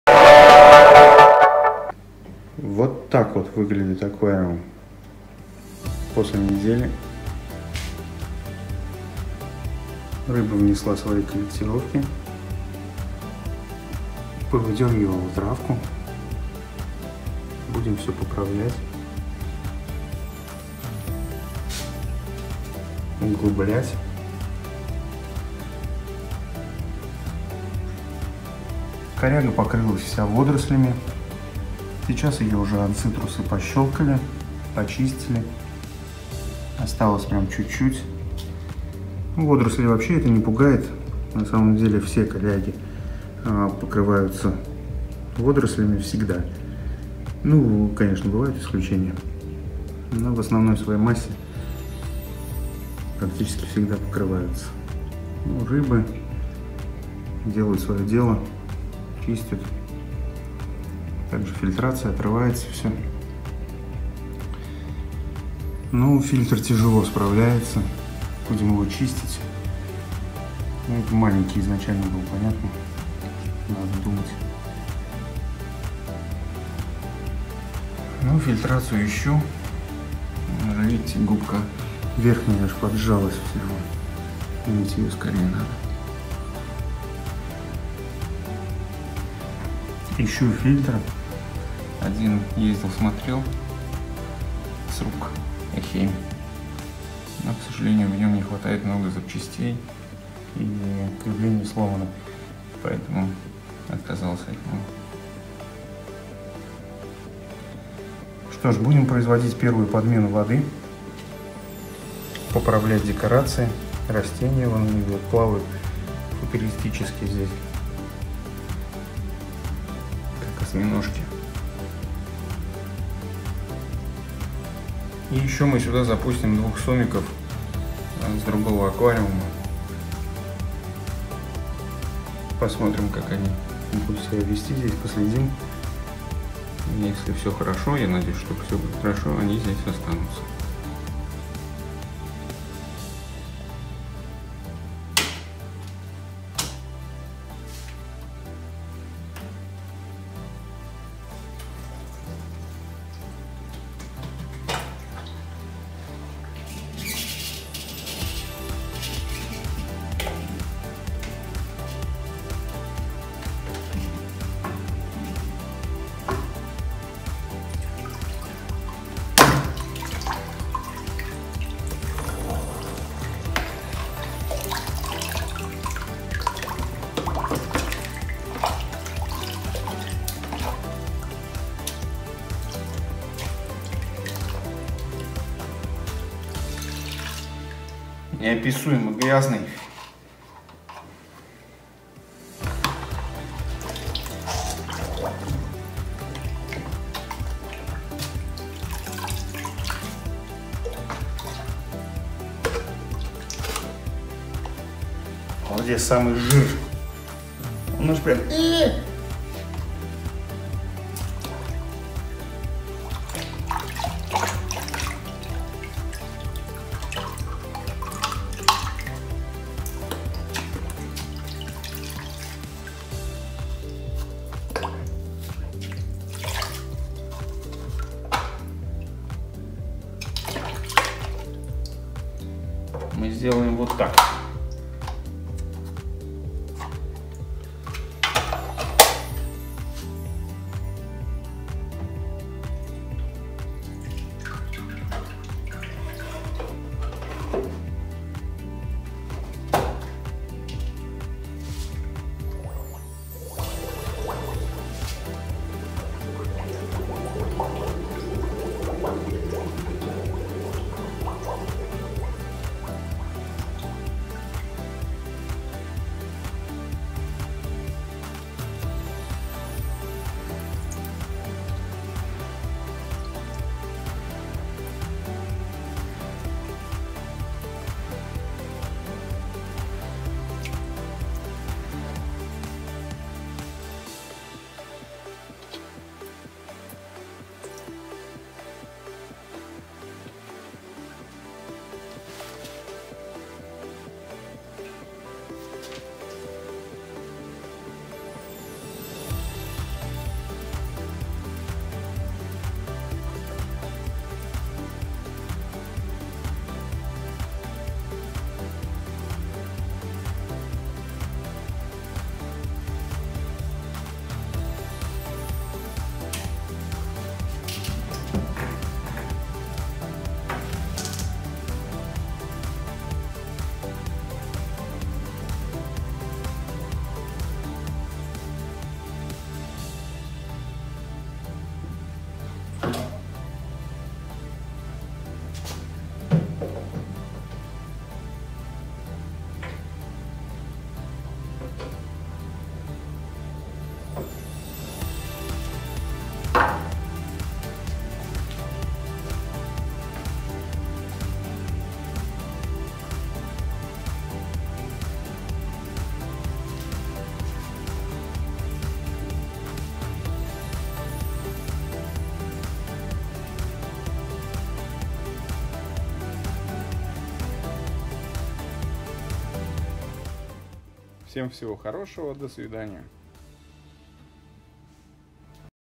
ОБСЛУЖИВАНИЕ 400 ЛИТРОВОГО АКВАРИУМА
спустя неделю после запуска аквариума я чищу фильтра, меняю 30% воды в аквариуме, поправляю декорации, исправляю ошибки сделанные при запуске аквариума в начале.